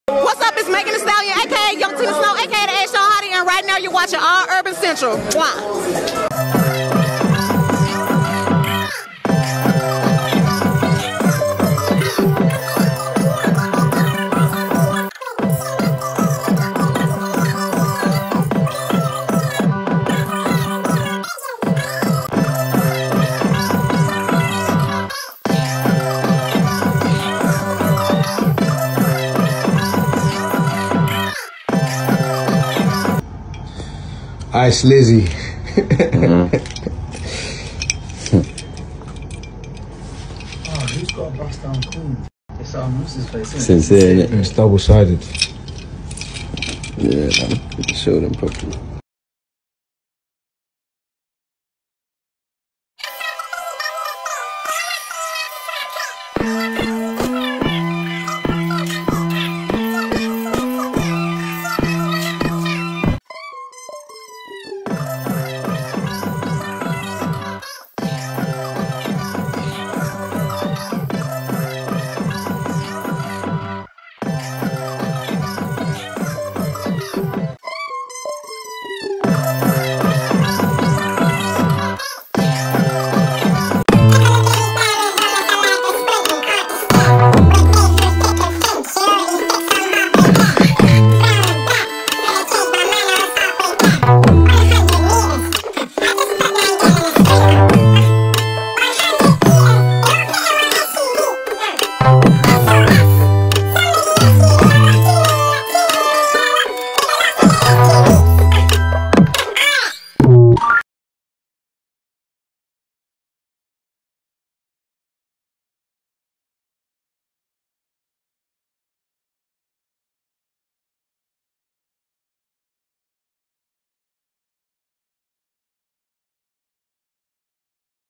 Ice Spice Trolls Central Cee's Girlfriend w Video Of Him In Bed With Her
Ice Spice and Central Cee Still Going Strong Amidst His Girlfriend Madaline Agry being upset